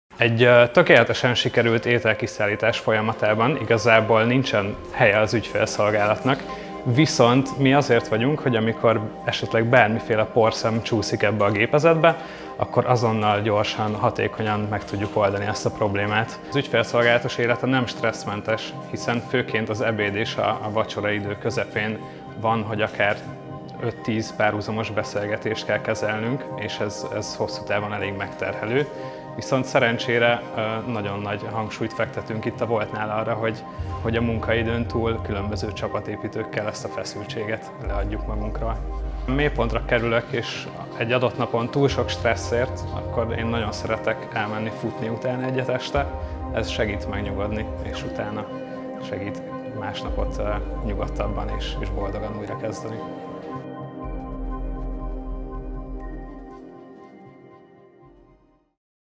Farkas Áron - ügyfélszolgálat, Wolt
Futár-világ
A Wolt célja, hogy minden perc maximálisan fel legyen töltve tartalommal és feladattal. 2018-ban hozták el Magyarországra azt a finn hátterű ételrendelő rendszert, ami újdonság volt a hazai piacon. Applikációjuk rendkívül erős felhasználói élményt nyújt valós idejű nyomonkövetéssel, magas szintű ügyfélszolgálattal és gyors panaszkezeléssel.